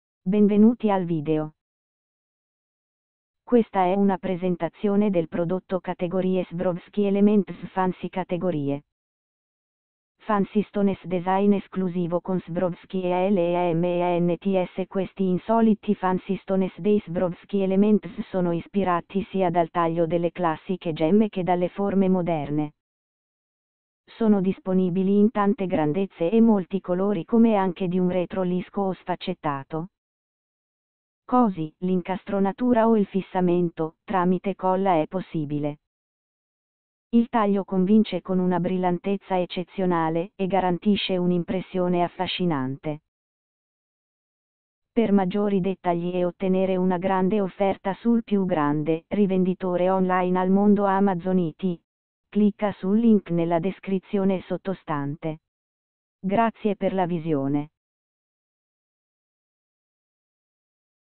Swarovski Elements Fancy Categorie
Swarovski Elements Fancy
Fancy Stones -- Design esclusivo con SWAROVSKI ELEMENTSQuesti insoliti Fancy Stones dei SWAROVSKI ELEMENTS sono ispirati sia dal taglio delle classiche gemme che dalle forme moderne. Sono disponibili in tante grandezze e molti colori come anche di un retro lisco o sfaccettato. Cosi' l'incastronatura o il fissamento tramite colla è possibile. Il taglio convince con una brillantezza eccezionale e garantisce un'impressione affascinante.